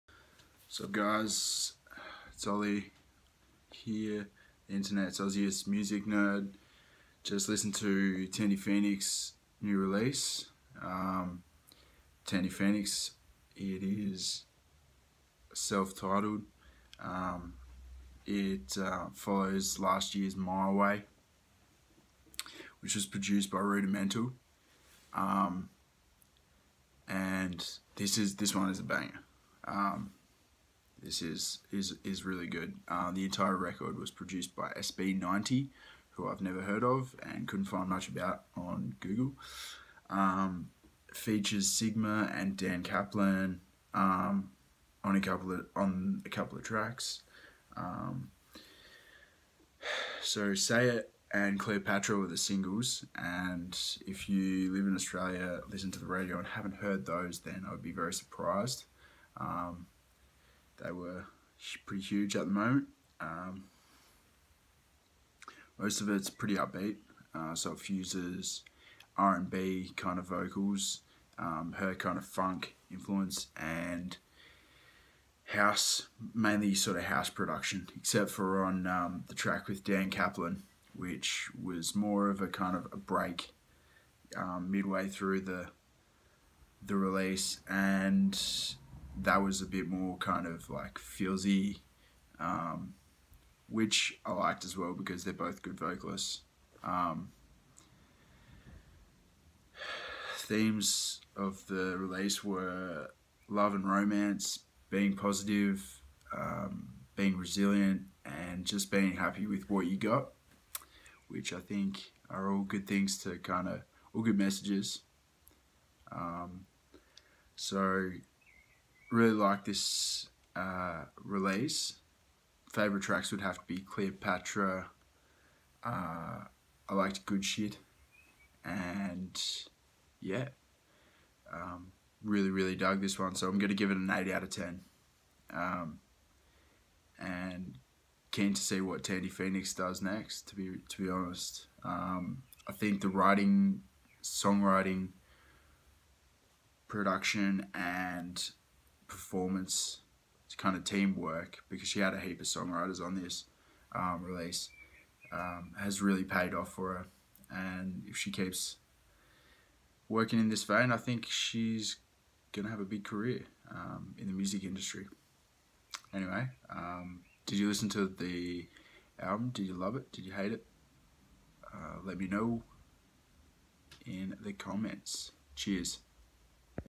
SWEATING MAN REVIEWS THANDI PHOENIX
It’s 39 degrees in Adelaide today and our study was an absolute sauna when I recorded this so please excuse my profuse sweating